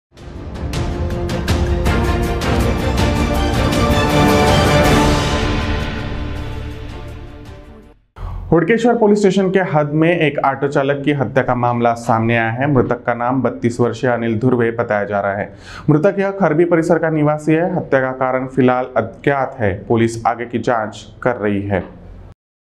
हुडकेश्वर मे आटो चालक की हत्या
मृतक 32 वर्षीय खरबी निवासी अनिल धुर्वे